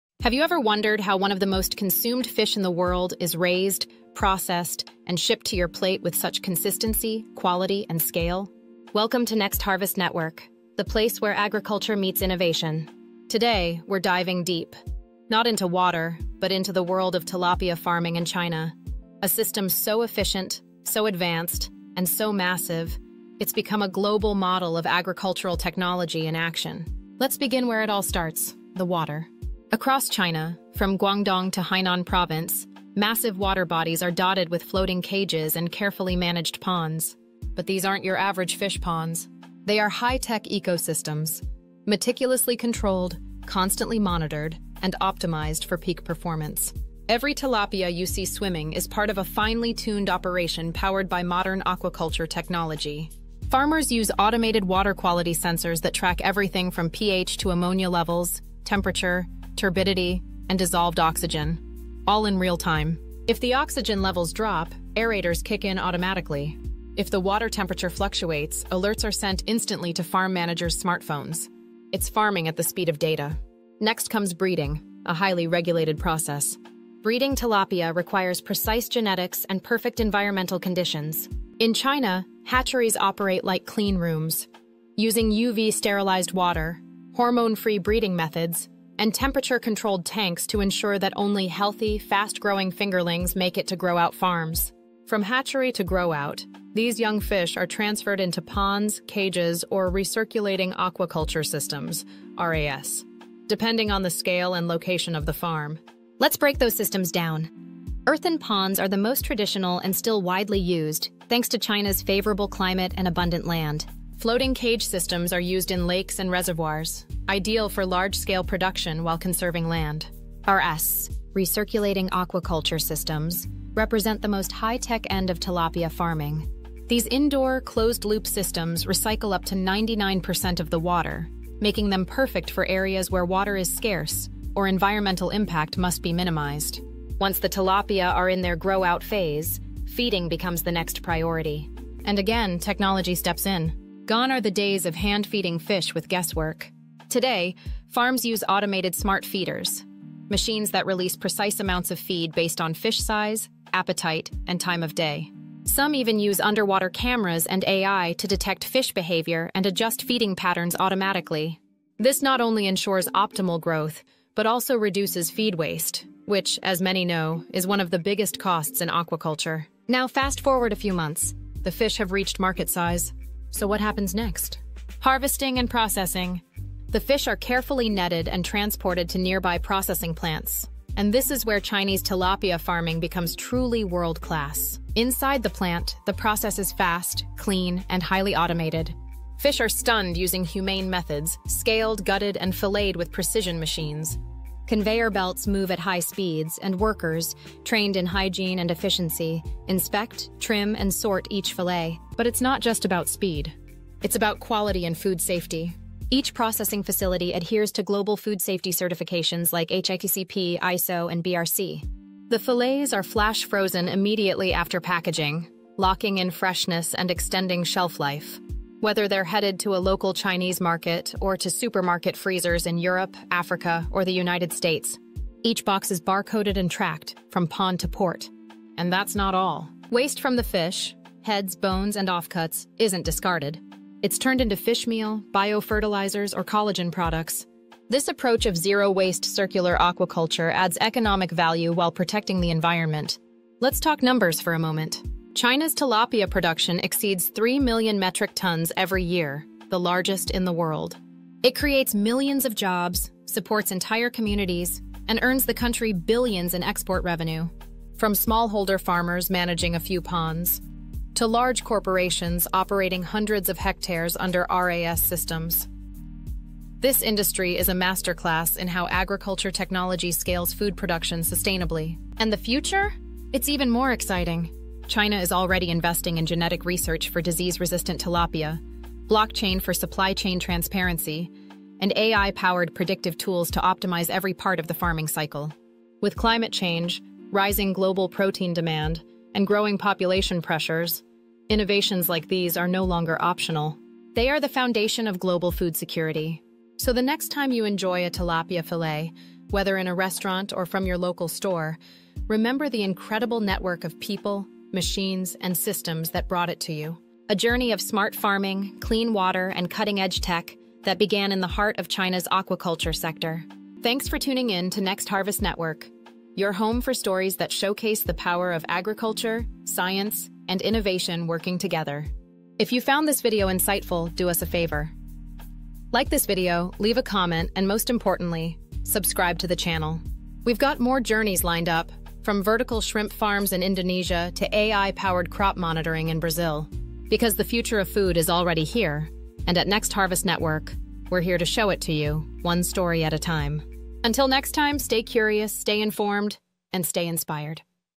Tilapia Farming in China Tech Revolutionizes Aquaculture!
China is transforming the fish farming industry — and the world is watching. In this groundbreaking episode from Next Harvest Network, we explore how advanced technology is revolutionizing tilapia farming, boosting productivity, improving water quality, and making aquaculture more efficient and profitable than ever before. From AI-powered feeding systems to fully automated smart ponds, China is proving that technology is the future of sustainable aquaculture.

This video reveals why China leads the world in aquaculture innovation, what technologies are behind their success, and how farmers around the globe — especially in Africa — can adapt these game-changing methods. Whether you're a farmer, agripreneur, or just passionate about the future of food, this story will inspire and inform you.

Join the movement. Be part of the next generation of smart farmers shaping the future of food security and agribusiness.